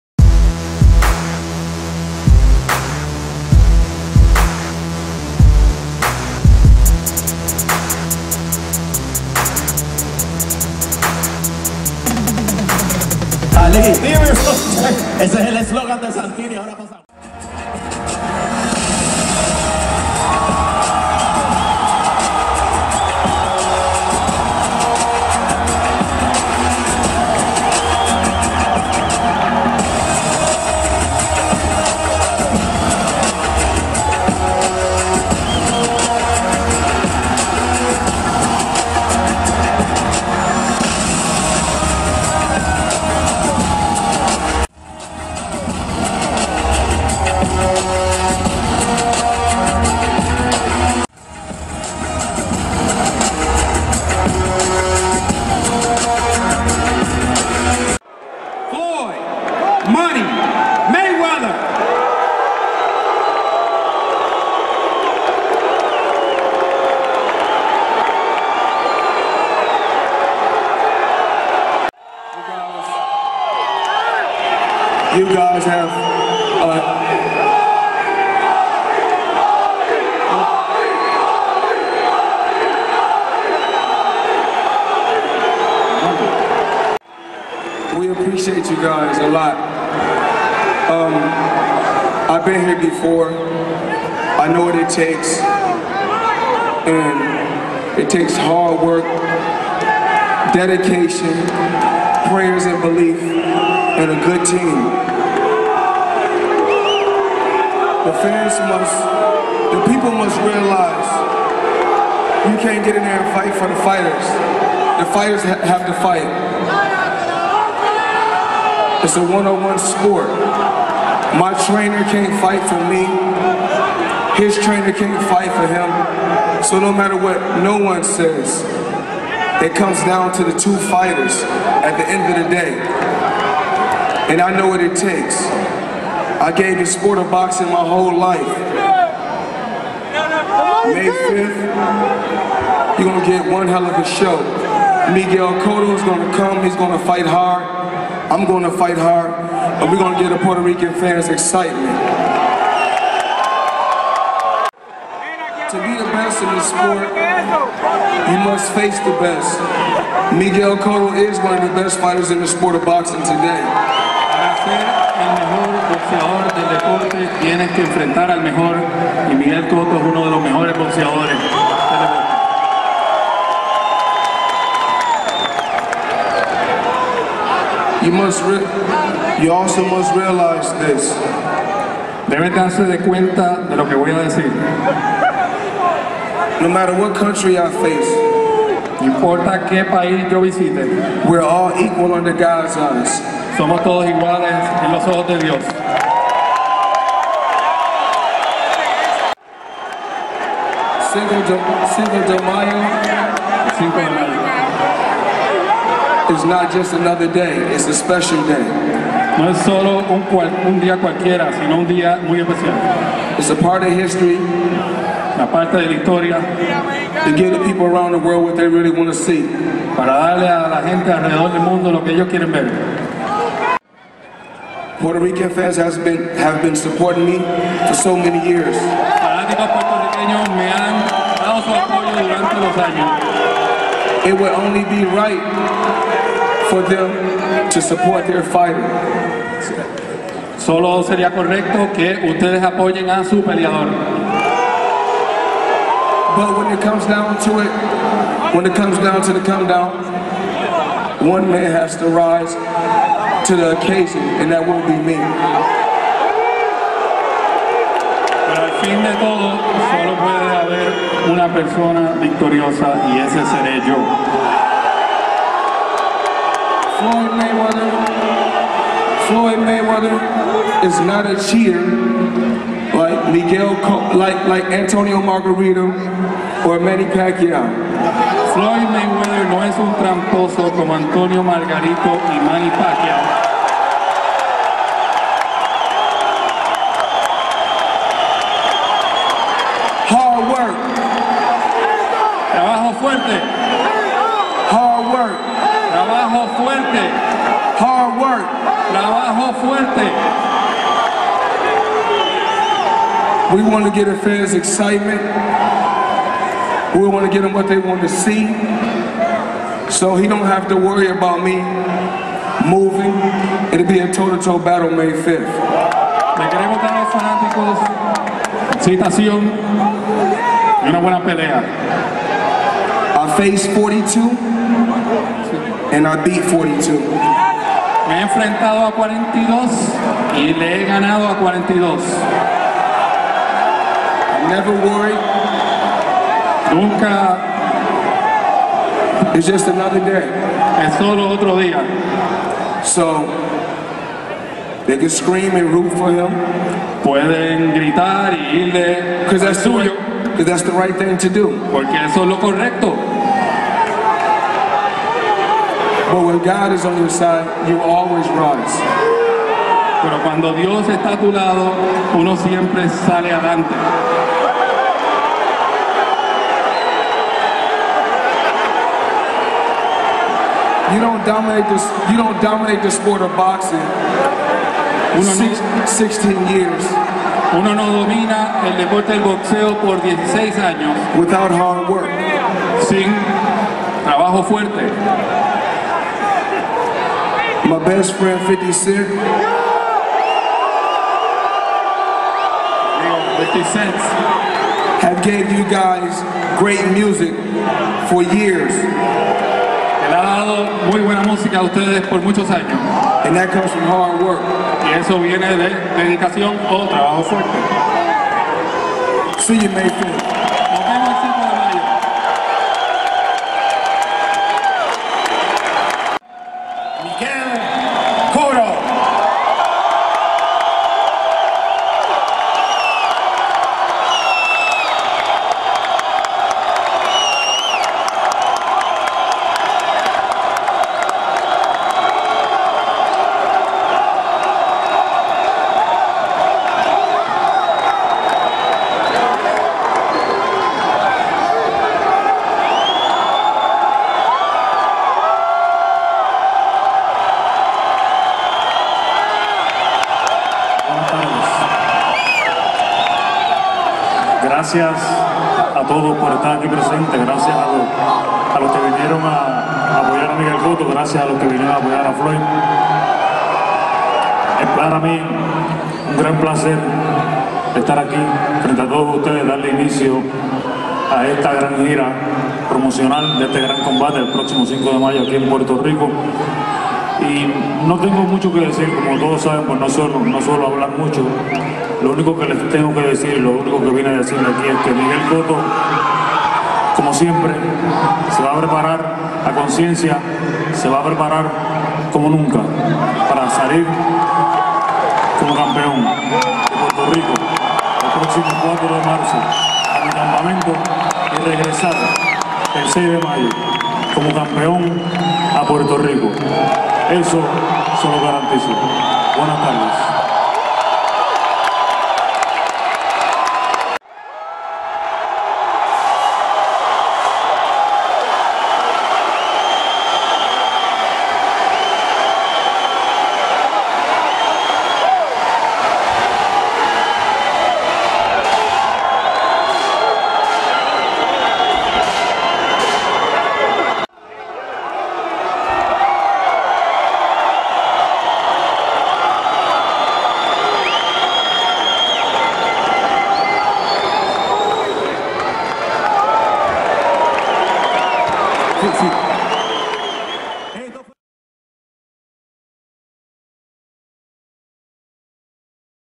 Miguel Cotto vs Floyd Mayweather Press Conference In Puerto Rico!
"Floyd Is Not A Cheater Like Antonio Margarito Or Manny Pacquiao"